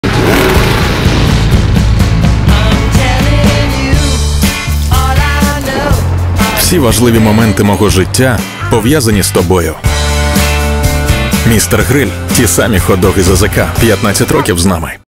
Mr.Grill. Ті самі хот-доги з АЗК
Ми презентуємо історію нашого продукту —  Mr.Grill. Це ті самі хот-доги з АЗК, на яких виросло вже не одне покоління і якість яких лишається незмінною ось уже понад 15 років!